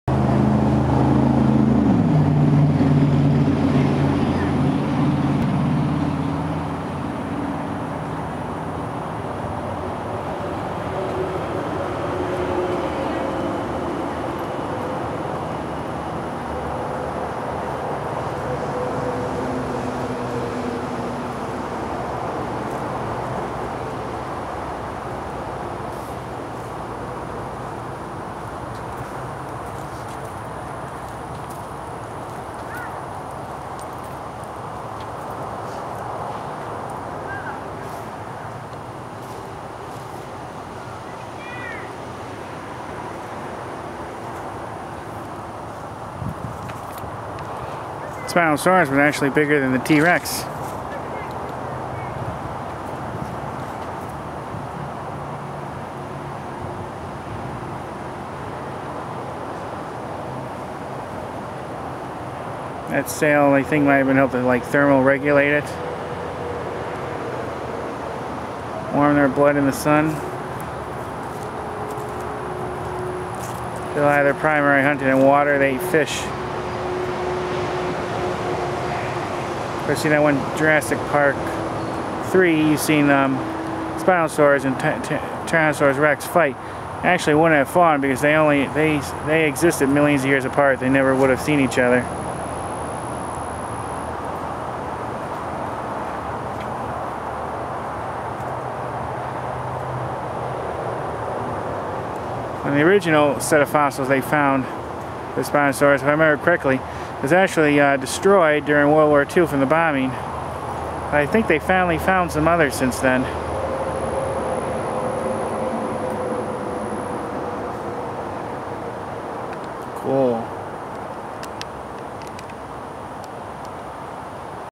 Spinosaurus at Dinosaur World FL
This video shows the Spinosaurus display at Dinosaur World FL. This was filmed on 12/10/2015.